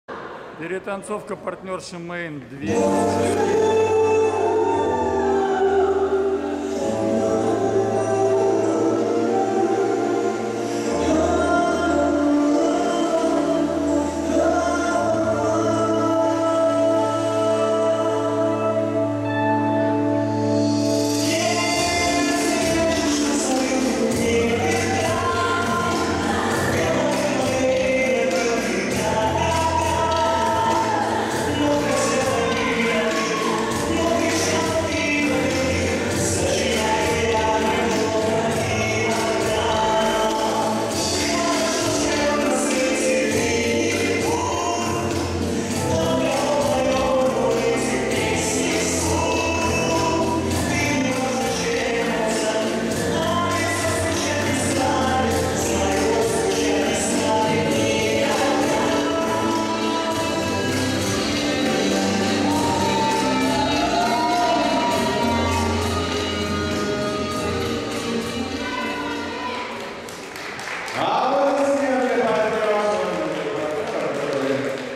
29.7.2018 MUC ReDance J&J Main партнерши проходит 2 из 6
Music Up Competition, рейтинговый турнир АСХ
Опубликовано: 30 сентября 2018 г.
Место проведения: в зале Карибия Данс Хол. Зеленый Проспект 10Б
Организатор: Международная школа танцев YouDance, Яна Степочкина
Непосредственные исполнители: Яна Степочкина, Андрей Степочкин и Ко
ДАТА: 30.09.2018
Официальный Видеооператор: Игорь Скобелев